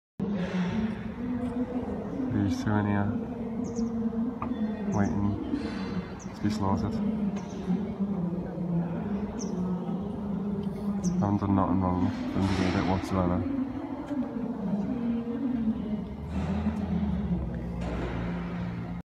Lonely COW in Holding Pen (at Slaughterhouse) 2 😢
Lonely COW in Holding Pen (at Slaughterhouse) 😢
This young cow is in a holding pen at a slaughterhouse, waiting for his turn to be killed. The holding pen is freezing cold, dark and smells terrible. Soon he will be herded out onto the kill floor to be shot in the head with a captive bolt gun, then be hoisted up by his leg and have his throat slit.

LOCATION: WIGAN, GREATER MANCHESTER
DATE: DECEMBER 2019
Footage shot by fellow activist